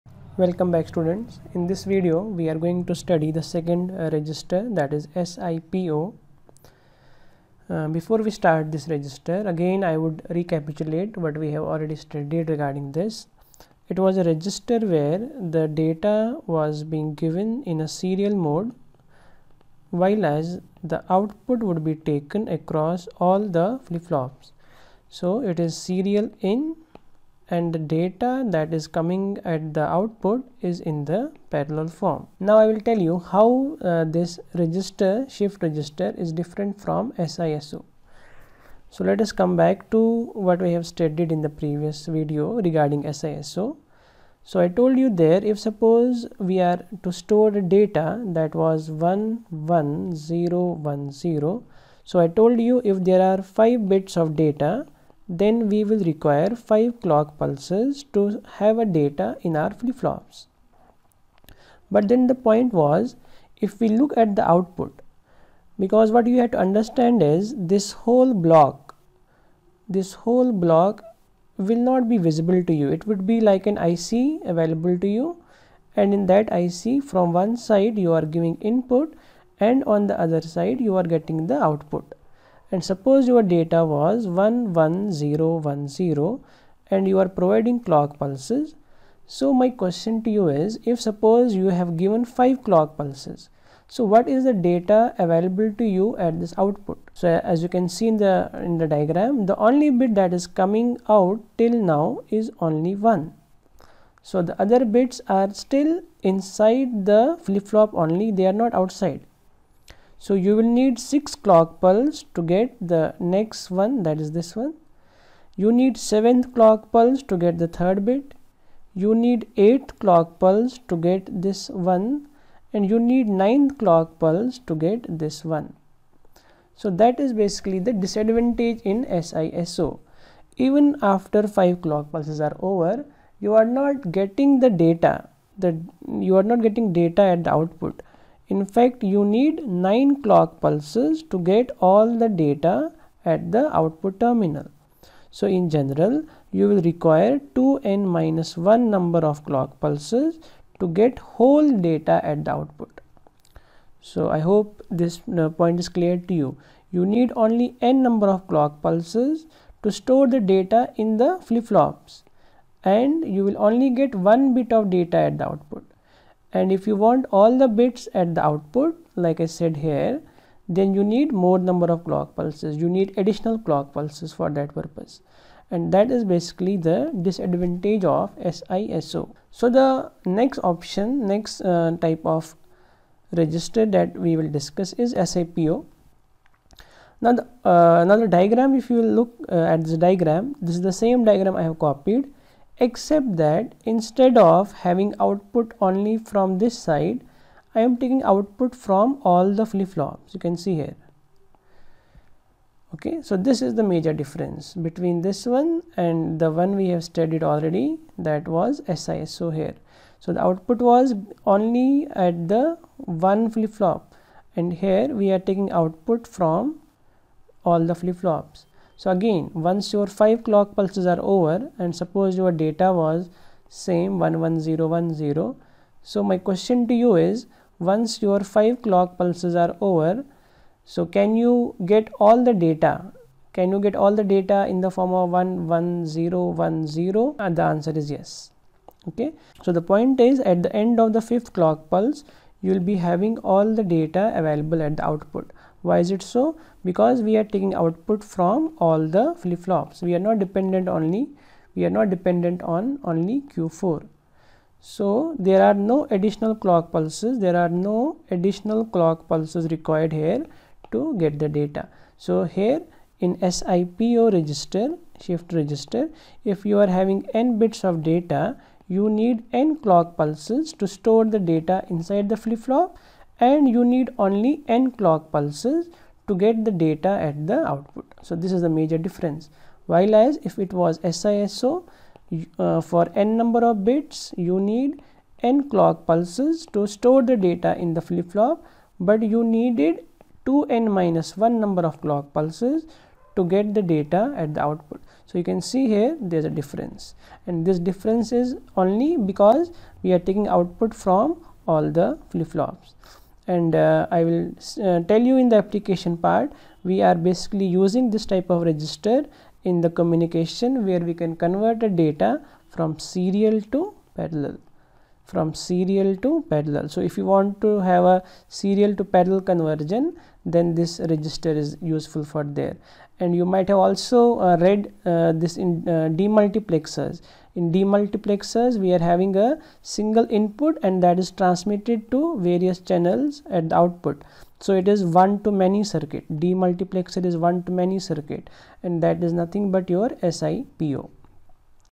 Serial In Parallel Out (SIPO) Shift Register
This video explains the shifting operation of SIPO register.